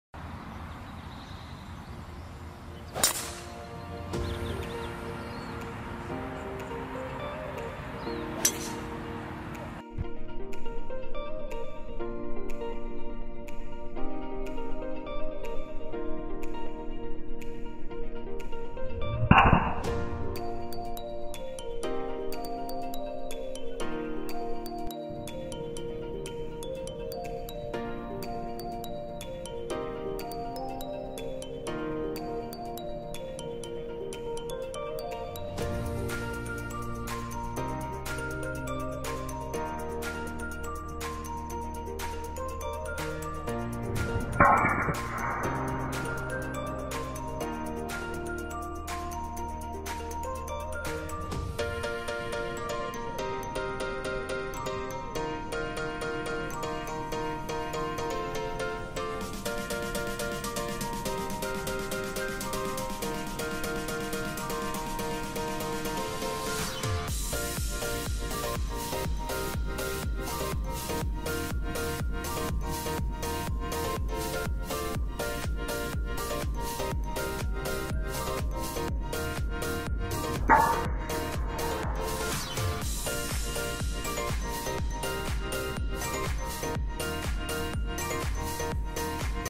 미녀 골퍼 "클라라 스필코바" 퍼펙트 스윙모션,Beauty Golfer _Klara Spilkova_ Perfect Swing Motion & Slow Motion
Beauty Golfer _Klara Spilkova_ Perfect Swing Motion & Slow Motion, 미녀 골퍼 "클라라 스필코바" 퍼펙트 스윙모션#Klaraspilkova